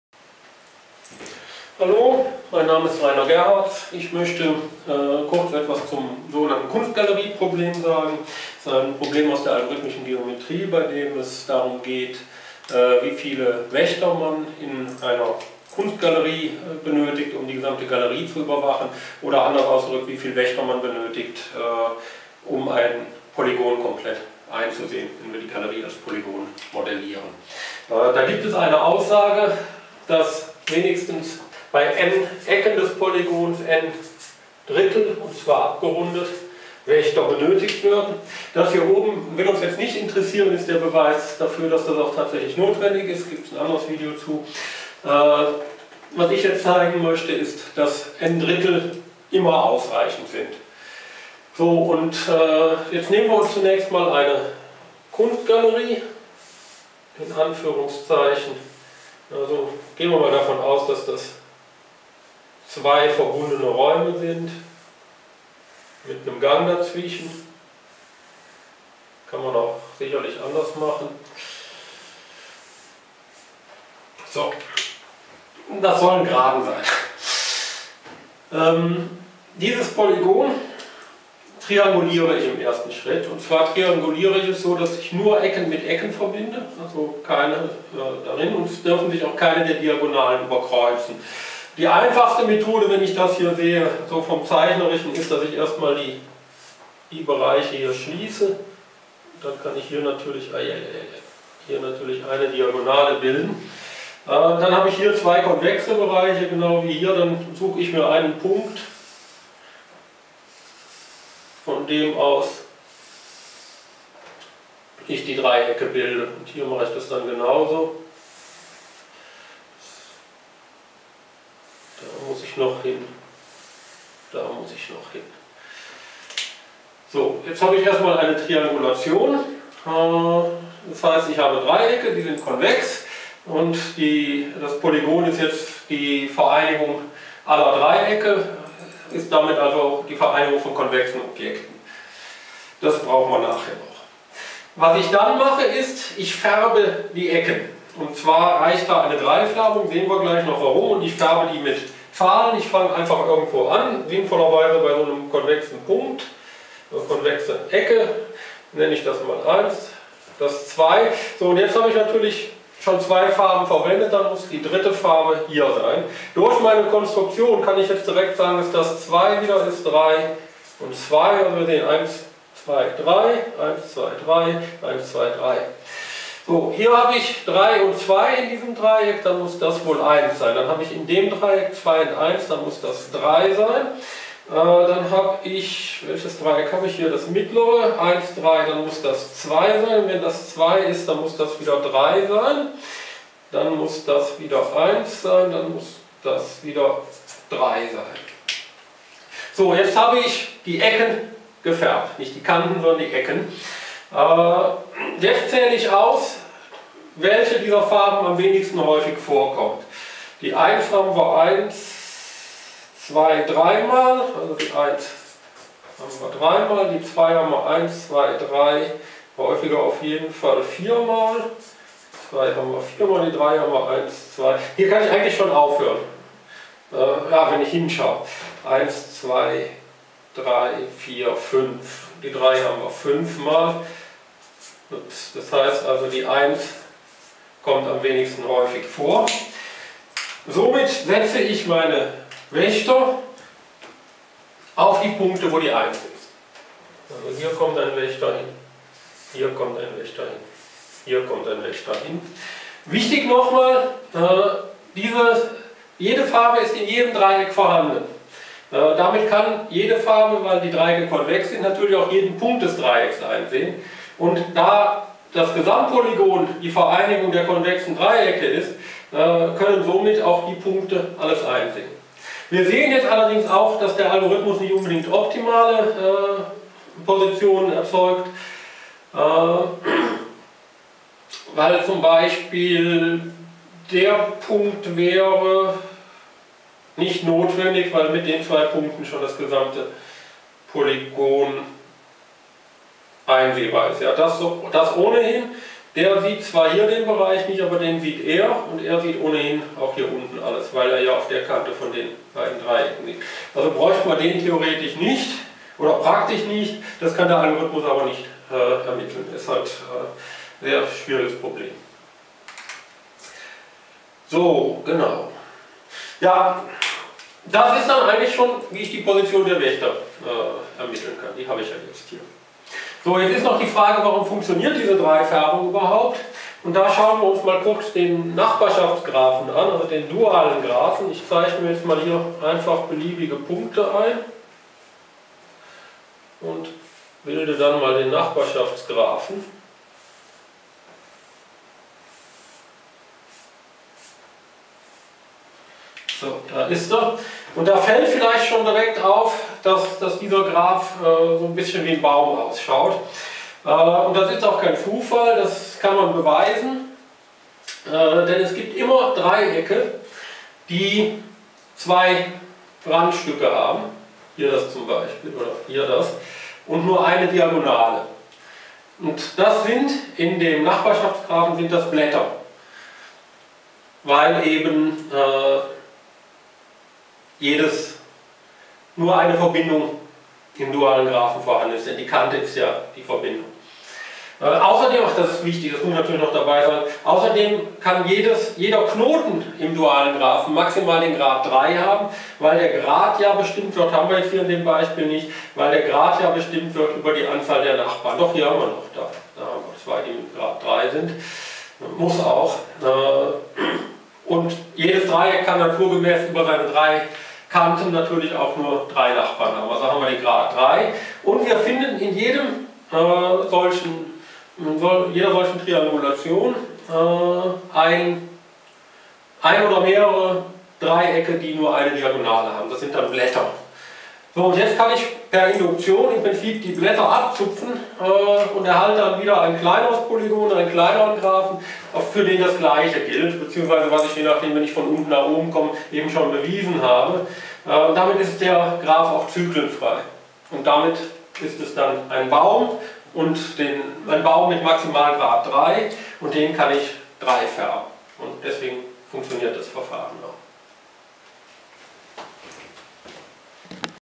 Kunstgalerieproblem: Wo werden die Wächter positioniert?
Beschreibung eines Verfahrens zur Positionierung der Wächter inklusive Beweis, dass n/3 viele immer ausreichen. Beweis der Notwendigkeit von n/3 Wächtern in